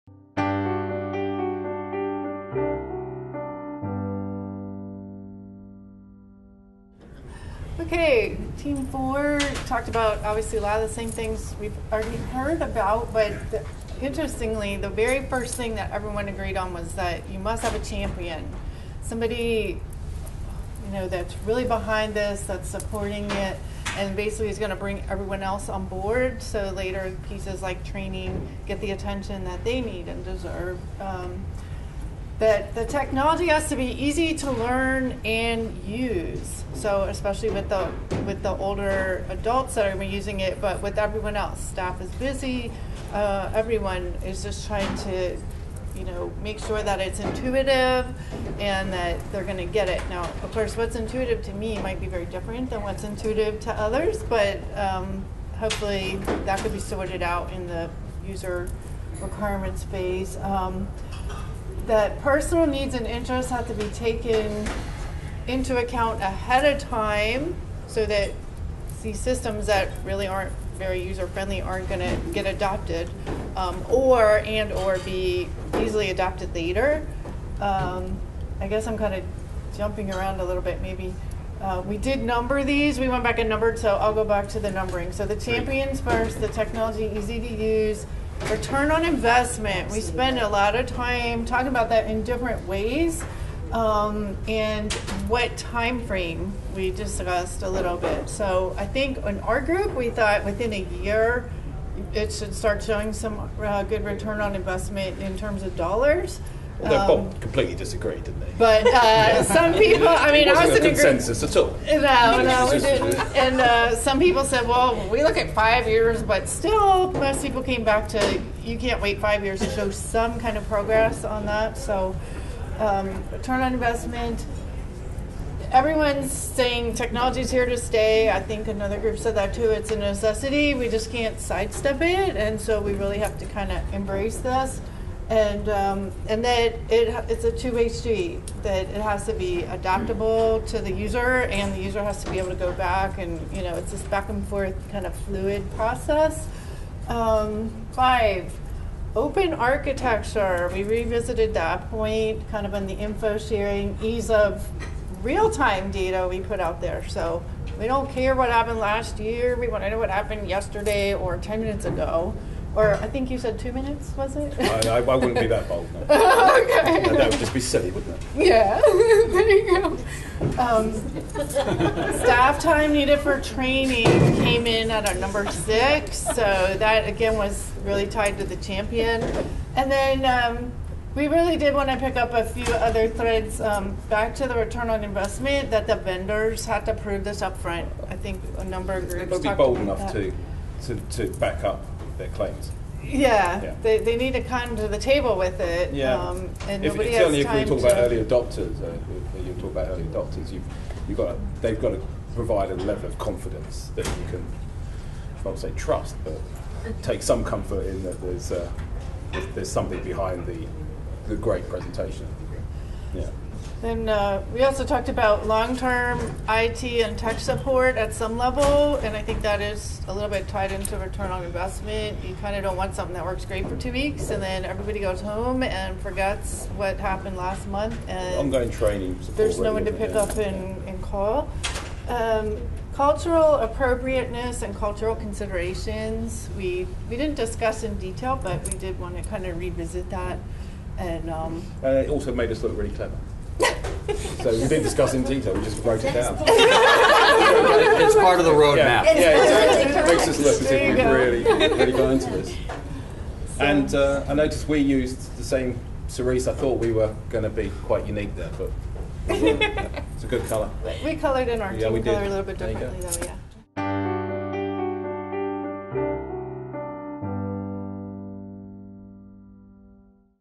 ICAA Forum 2016: The future of technology for active aging part 7
Team 4: ICAA Forum participants three key takeaways from the day.